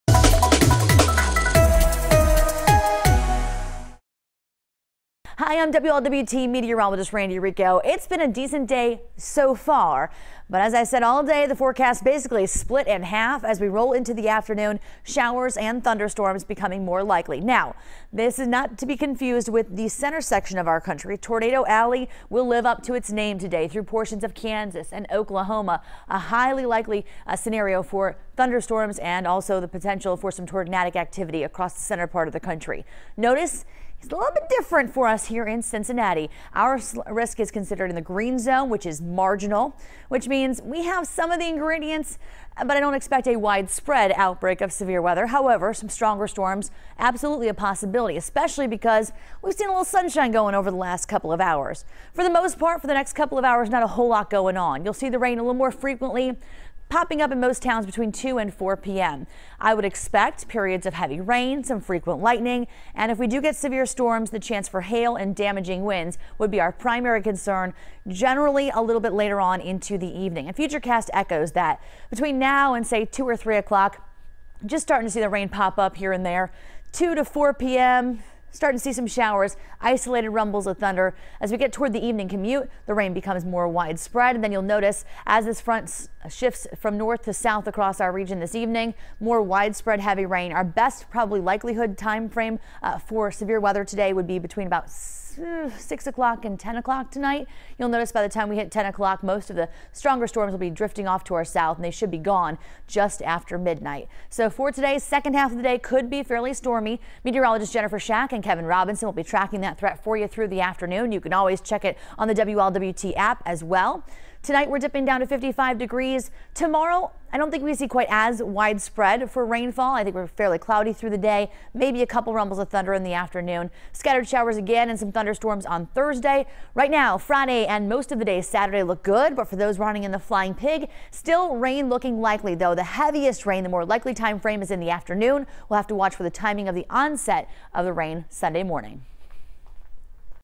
Storms Flare This Afternoon
Storms which could be on the strong side expected into the evening.